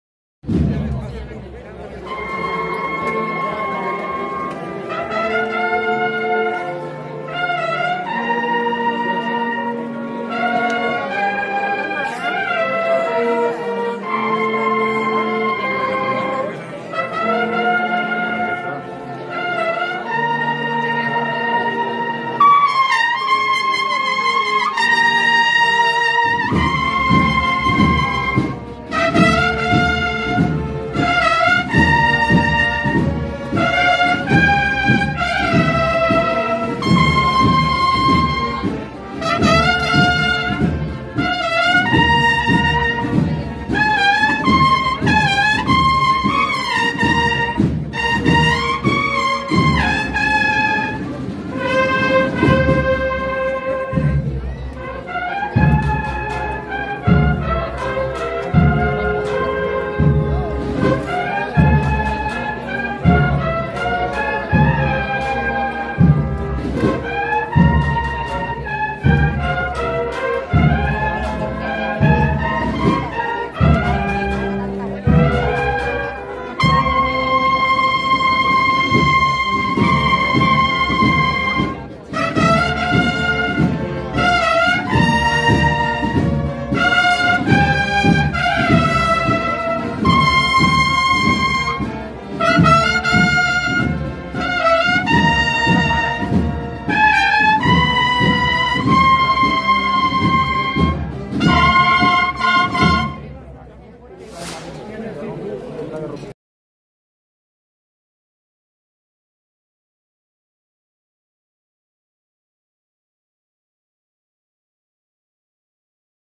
Sones de Sol - BCT Ntra Sra del Sol en Aznalcazar 2024 (Fiestas de la Encarnación)
📽Bando anunciador de las fiestas en honor a Nuestra Señora de la Encarnación  con los sones de la Banda de Cornetas y Tambores Nuestra Señora del SôL de Sevilla.

📍Aznalcazar (Sevilla).

✖️ X.